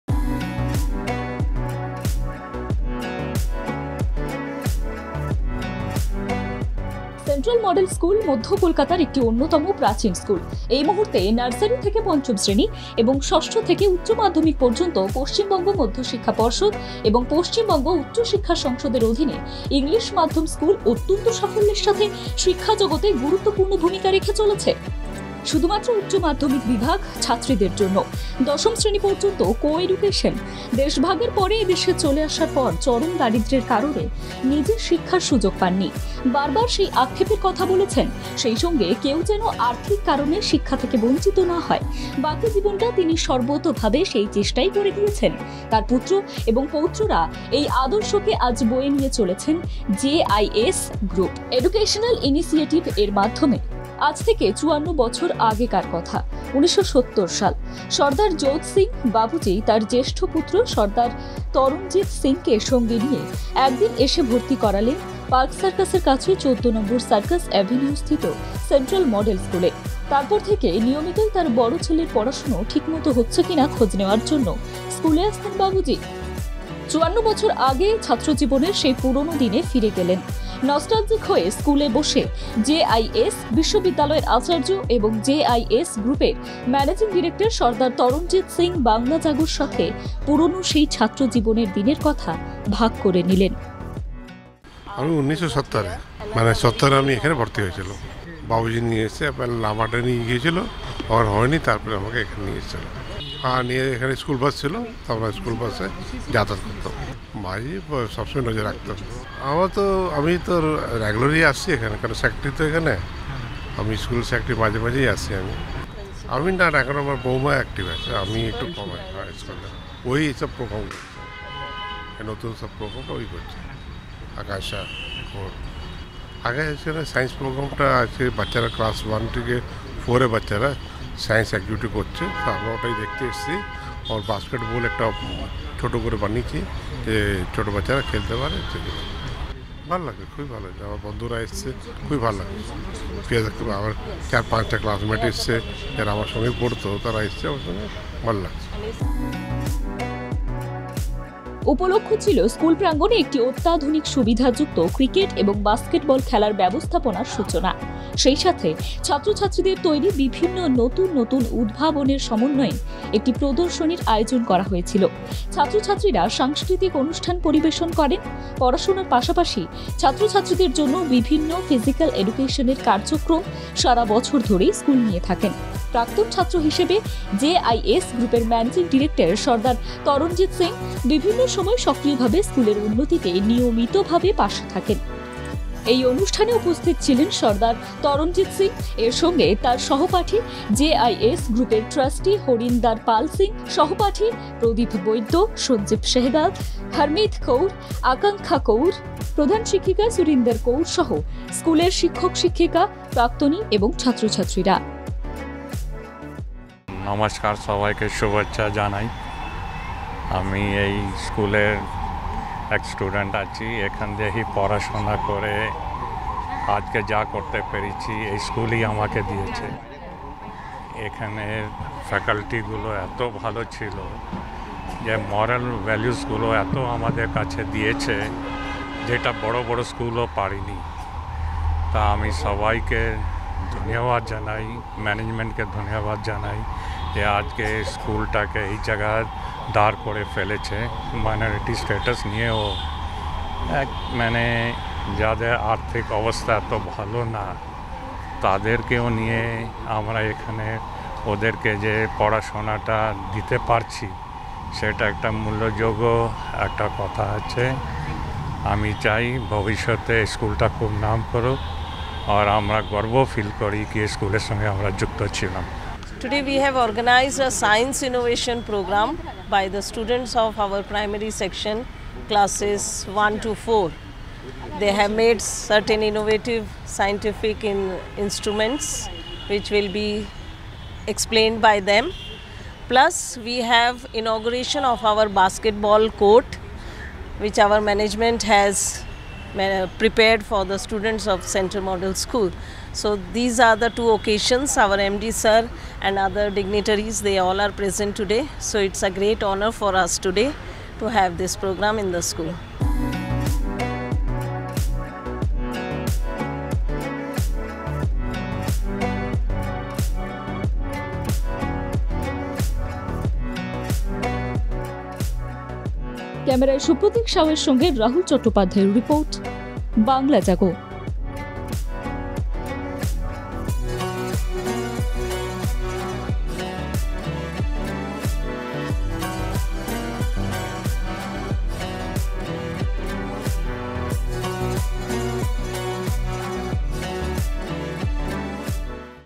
JIS : সেন্ট্রাল মডেল স্কুল মধ্য কলকাতার একটি অন্যতম প্রাচীন স্কুল
সেন্ট্রাল মডেল স্কুল মধ্য কলকাতার একটি অন্যতম প্রাচীন স্কুল।এই মুহূর্তে নার্সারি থেকে পঞ্চম শ্রেণী এবং ষষ্ঠ থেকে উচ্চ মাধ্যমিক পর্যন্ত পশ্চিমবঙ্গ মধ্যশিক্ষা পর্ষদ ও পশ্চিমবঙ্গ উচ্চ শিক্ষা সংসদের অধীনে ইংলিশ মাধ্যম স্কুল অত্যন্ত সাফল্যের সাথে শিক্ষা জগতে গুরুত্বপূর্ণ ভূমিকা রেখে চলেছে।শুধুমাত্র উচ্চ মাধ্যমিক বিভাগ, ছাত্রীদের জন্য।